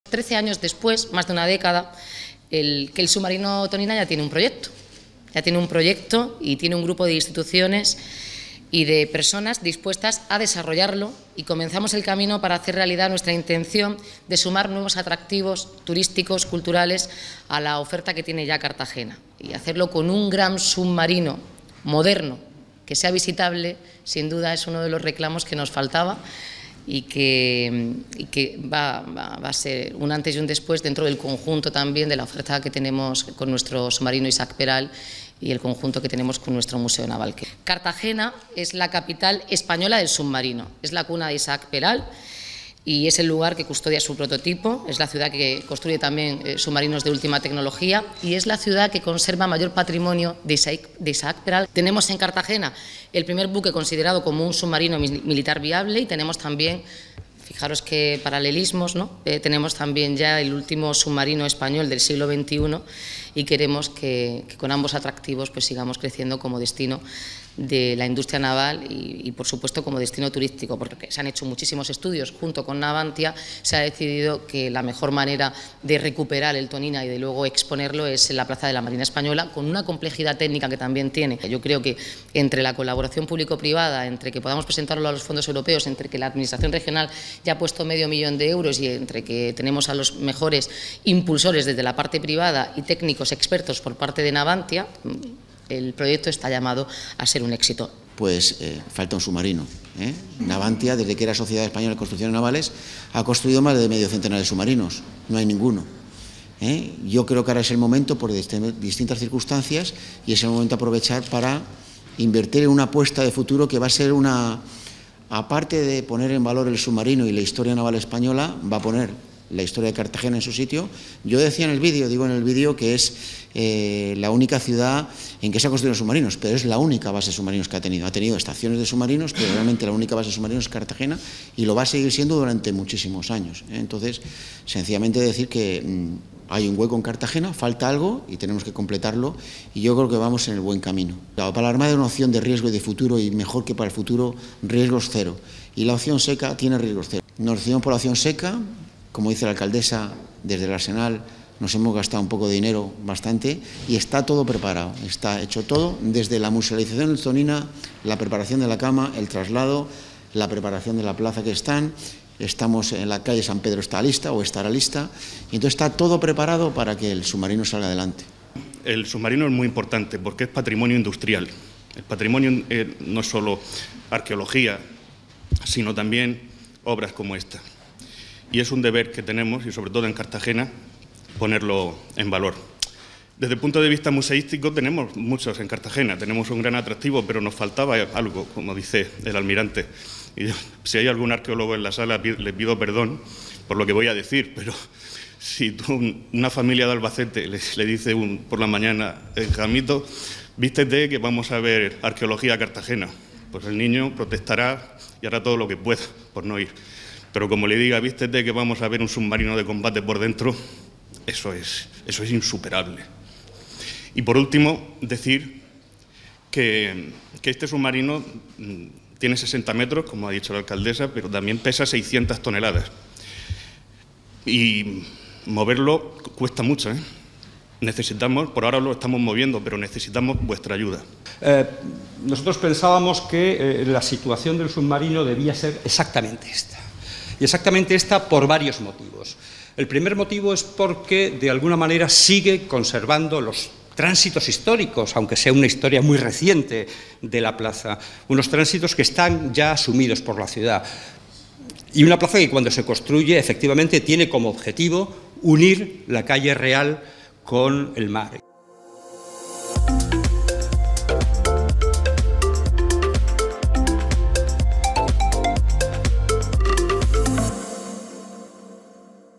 Cartagena recupera el submarino Tonina, que se expondrá en seco en la plaza de la Marina Española
El submarino Tonina ya tiene proyecto de musealización y se exhibirá en seco en la plaza del CIM, según ha anunciado alcaldesa, Noelia Arroyo, durante el acto de presentación del proyecto de exhibición del sumergible cedido por la Armada Española a la ciudad de Cartagena, en el que ha participado el almirante del Arsenal, Pedro Luis de la Puente, impulsor de la iniciativa; el director general de Patrimonio Cultural, Pablo Braquehais, y el arquitecto Andrés Cánovas, responsable del proyecto de adecuación de la plaza.

La alcaldesa ha manifestado que la idea inicial era que el Tonina debía exhibirse junto al Museo Naval de Cartagena para formar un conjunto con el prototipo de Isaac Peral, pero que el debate técnico se ha centrado en decidir si era más conveniente exponer el submarino en el agua, en la dársena de botes, o hacerlo en seco en la plaza de la Marina Española.

Al final, la decisión se toma después de reunir todos los datos posibles. En la dársena se hicieron estudios hidrográficos y estudios con sonar; se ha analizado la resistencia de la plaza con estudios geotécnicos para conocer su resistencia, y desde Navantia se han estudiado las opciones de traslado y de su musealización. Y todo bajo la supervisión de la Dirección General de Patrimonio Cultural.

La decisión final ha sido que el submarino debe exponerse en seco en la plaza de la Marina Española. “Esa es la fórmula escogida para la mayoría de los submarinos expuestos en el mundo porque garantiza mejor su conservación y su mantenimiento y esa debe ser una prioridad”, ha dicho Arroyo.

El proyecto está valorado en algo más de tres millones de euros, de los que sólo el desplazamiento supone en torno al millón de euros; el refuerzo de la plaza ronda el medio millón de euros y el resto es el coste estimado de la preparación del submarino para su visita.

La alcaldesa ha recordado que ya se dispone de los primeros 500.000 euros para el proyecto que el Gobierno Regional ha previsto en sus presupuestos de este año para costear la obra de la plaza. Y desde este mismo momento se van a empezar a captar fondos de empresas y de las administraciones para completar el presupuesto.

“Hoy nos han querido acompañar líderes empresariales que ya nos han manifestado su compromiso de unirse a nosotros en la gestión de esa financiación y convertir el proyecto en un ejemplo de colaboración público-privada. Porque todos tenemos el convencimiento de que este proyecto impulsará nuestro desarrollo como destino turístico”, ha dicho la alcaldesa.